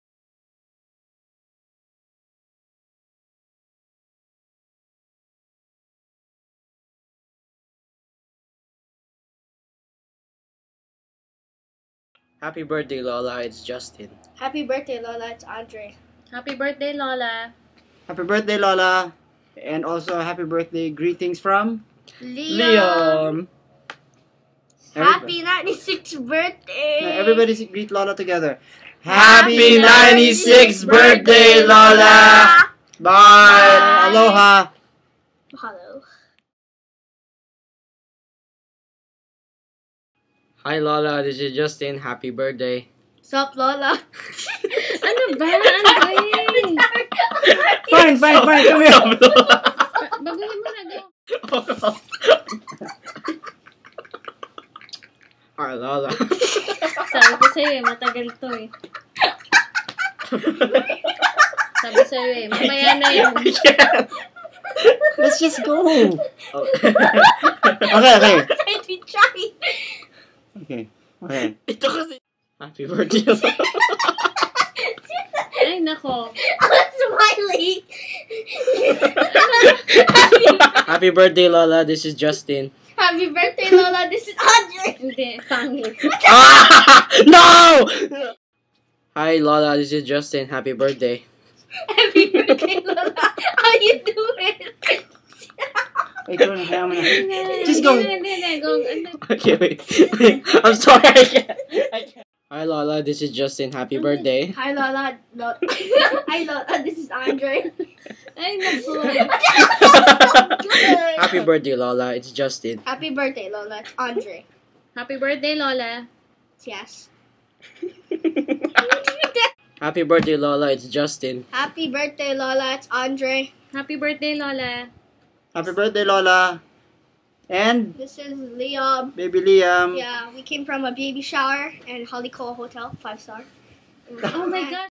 Happy 96th Birthday Lola!
birthday greetings for our 96 year old grandmother.